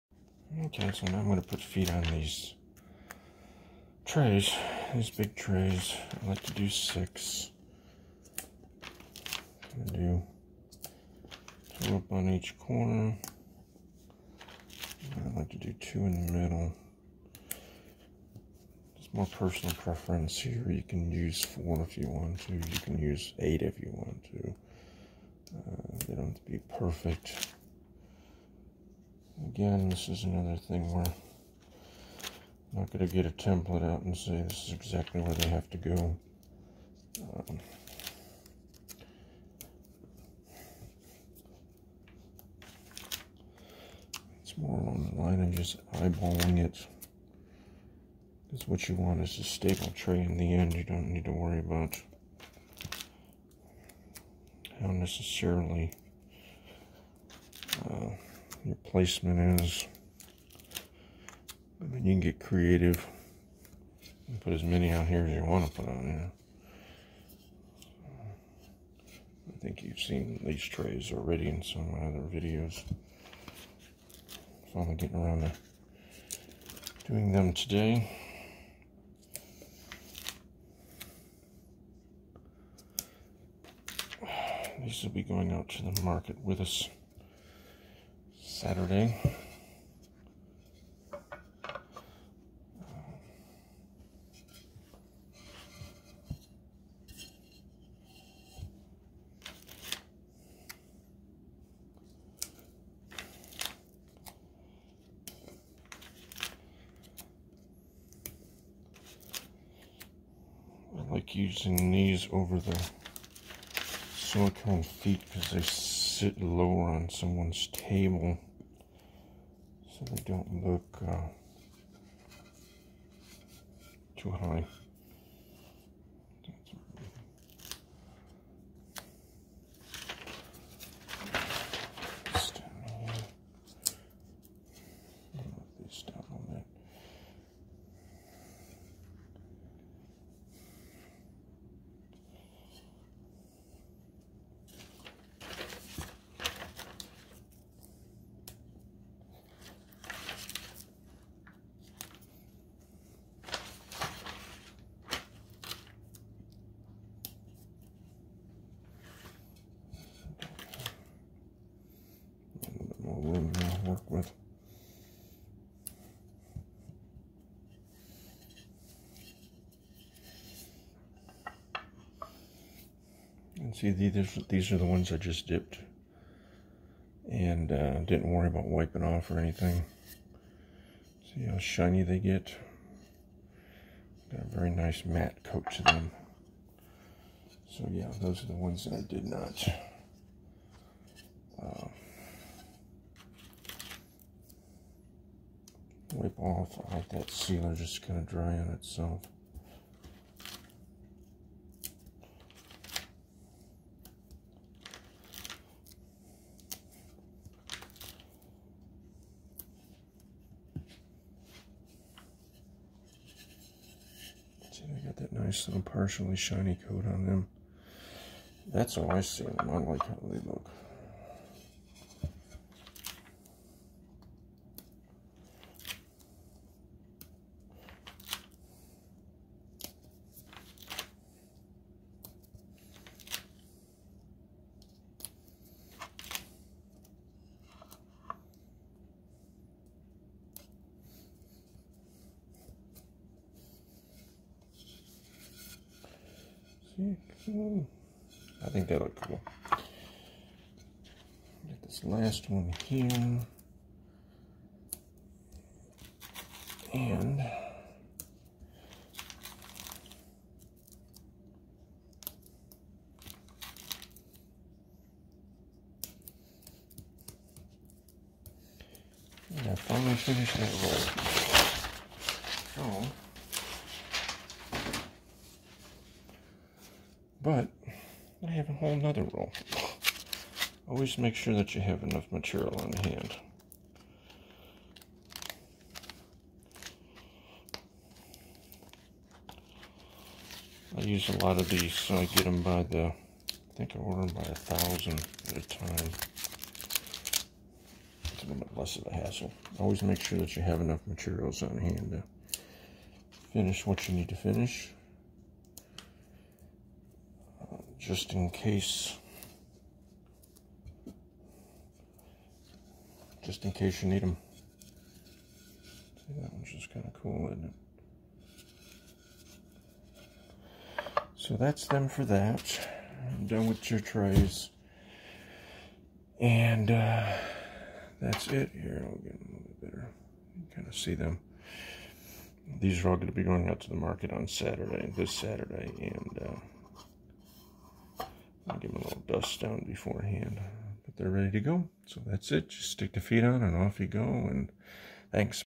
Putting Cork Feet on the new Big Trays from this week.
These will be available at the Market this weekend and on our website next week.

Texas Nature Crafts and Stones Texasnaturecraftsandstones.com